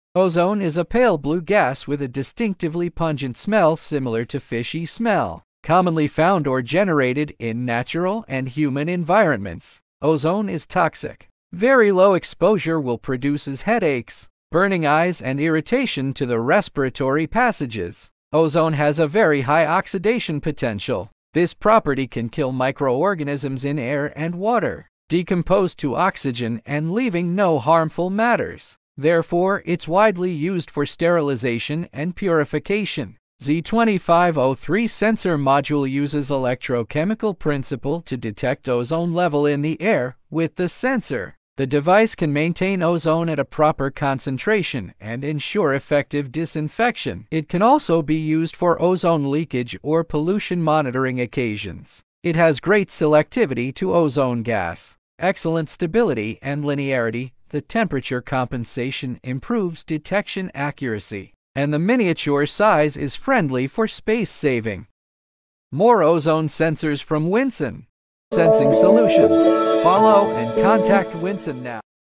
Winsen Ozone O3 Gas Sensors
ZE25-O3 sensor module uses electrochemical principle to detect ozone level in the air. With the  sensor, the device can maintain a proper O3 concentration and ensure effective disinfection.

It’s widely used for sterilization and purification of:
- closed air space, municipal water, industrial sewage, medical device, food industry, aquaculture breeding, etc.
- household appliances like washer, shoe cabinet, air purifier, water purifier...
- ozone leakage or pollution monitoring occasions : O3 generators, offices with photocopiers, laser printers, production process...

It has great selectivity to ozone, excellent stability and linearity, temperature compensation for high accuracy, miniature size for good integration.